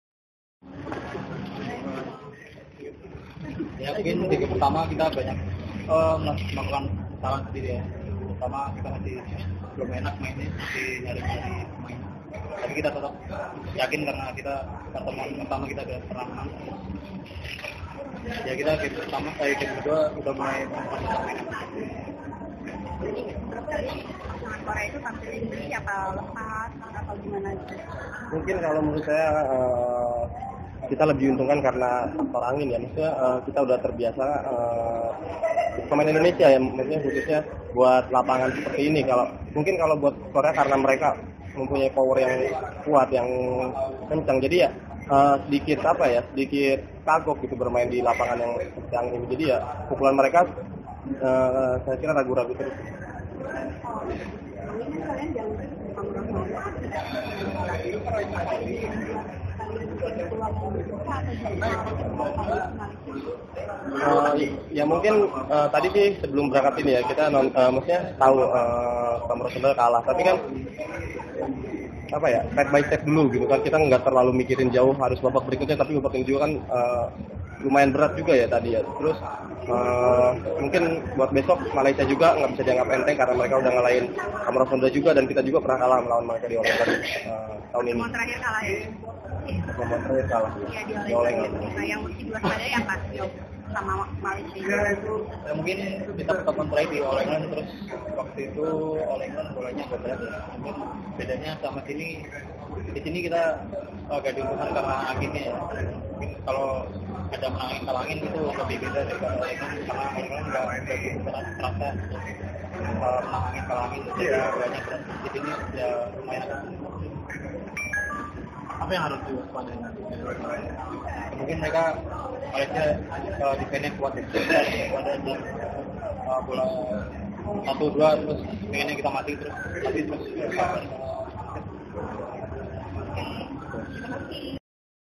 [Rekaman Suara] Fajar/Rian Usai Kalahkan Kim Won Ho/Seo Seung Jae di Babak 16 Besar Asian Games 2018
Sumber Foto : PBSI